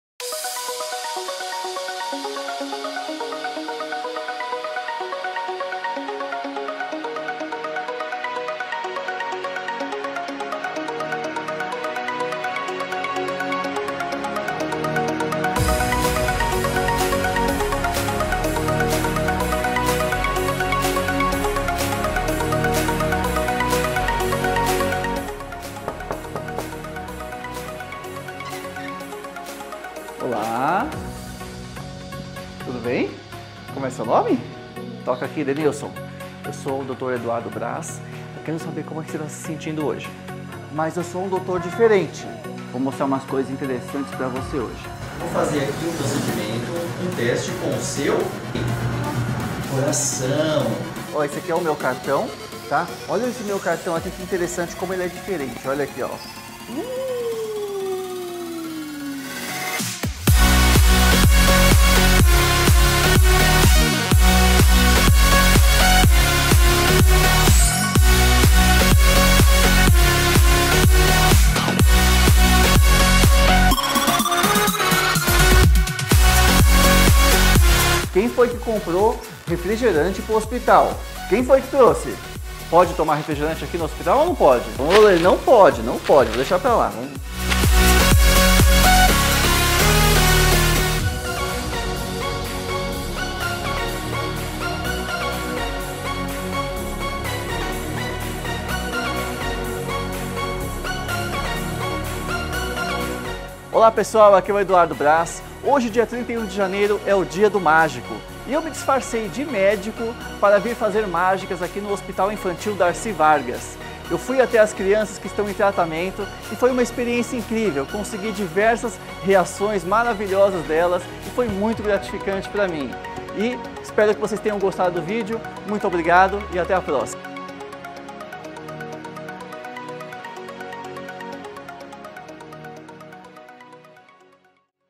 Mágico arranca sorrisos de crianças internadas
Você pode pensar que o dia a dia de um hospital infantil é sempre igual, mas na verdade ele pode ser mágico ✨